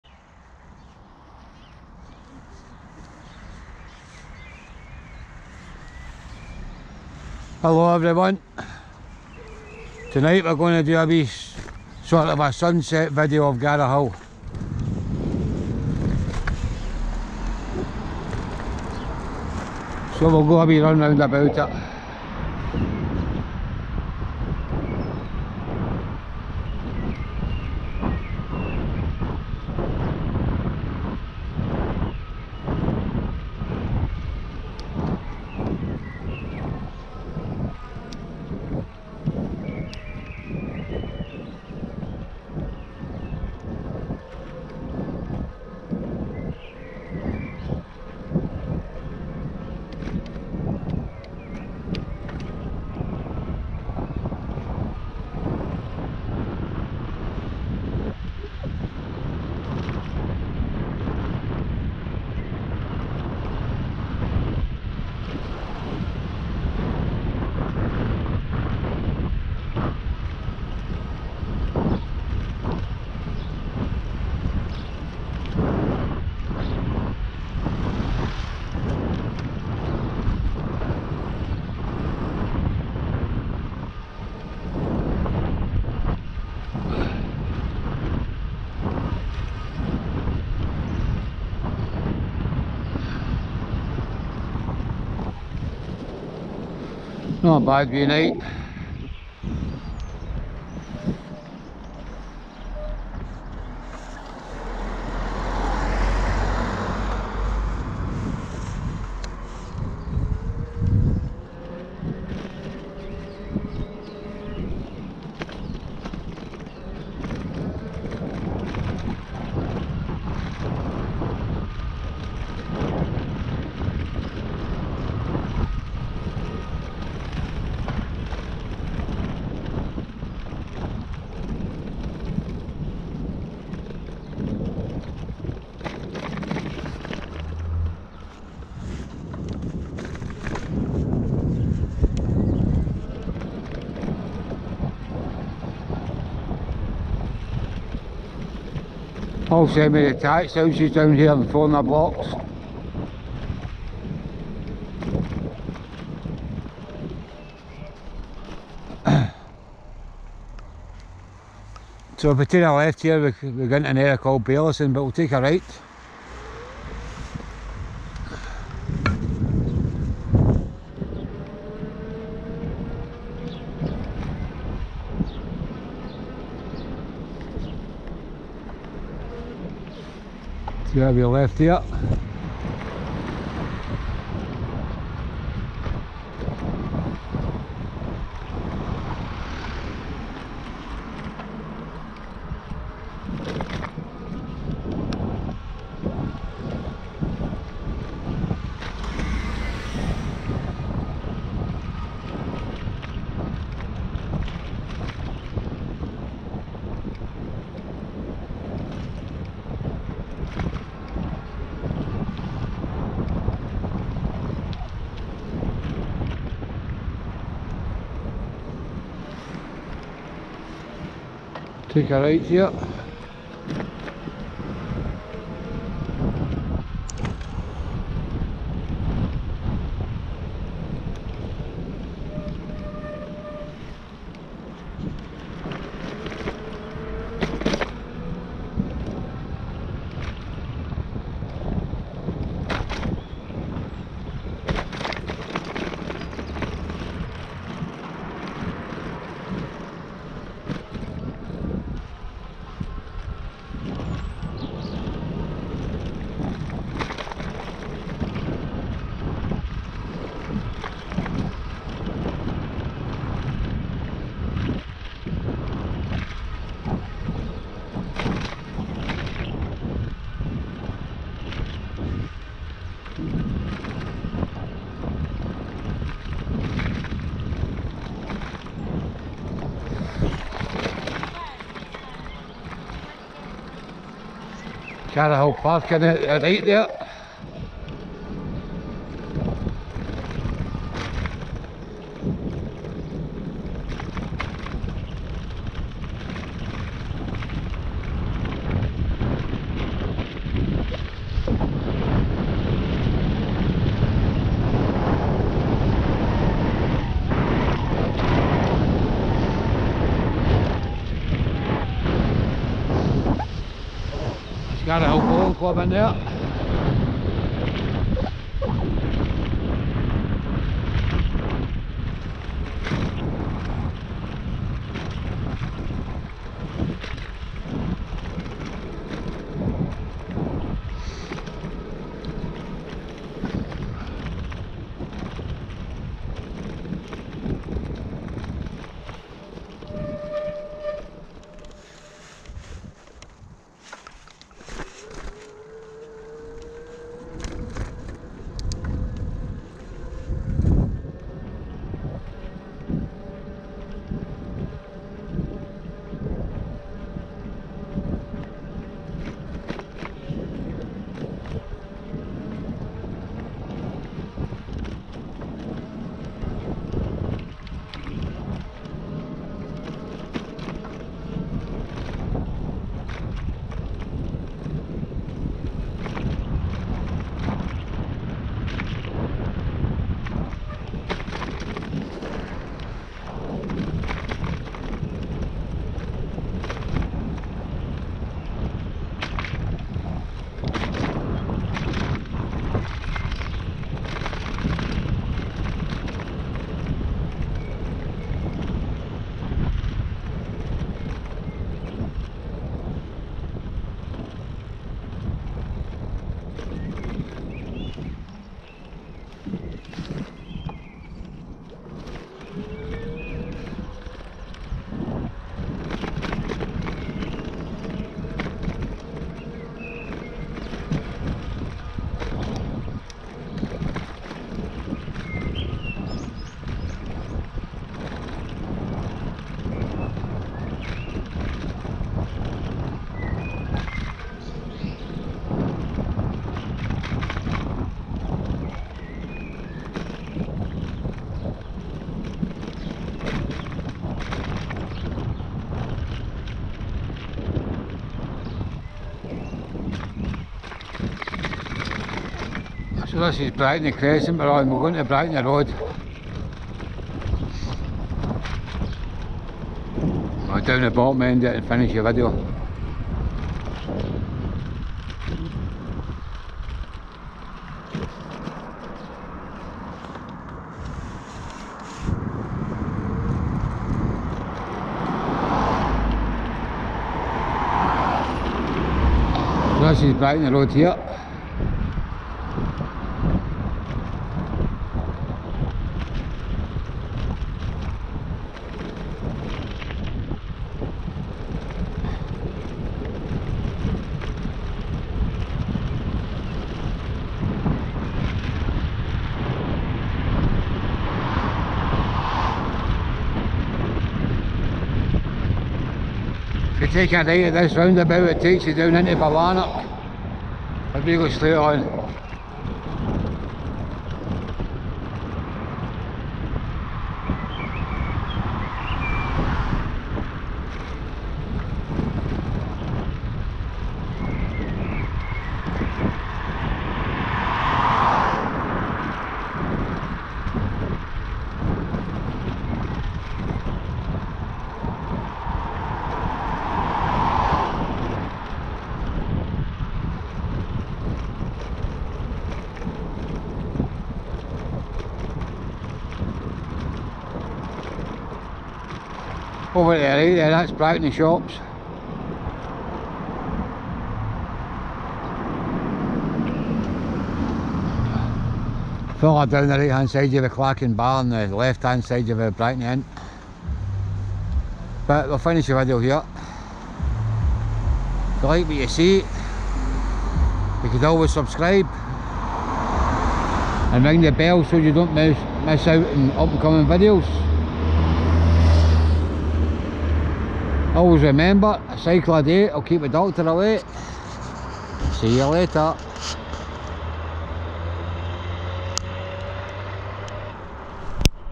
GLASGOW CYCLE EAST END (GARROWHILL)
Cycle around Garrowhill, in the east end of Glasgow.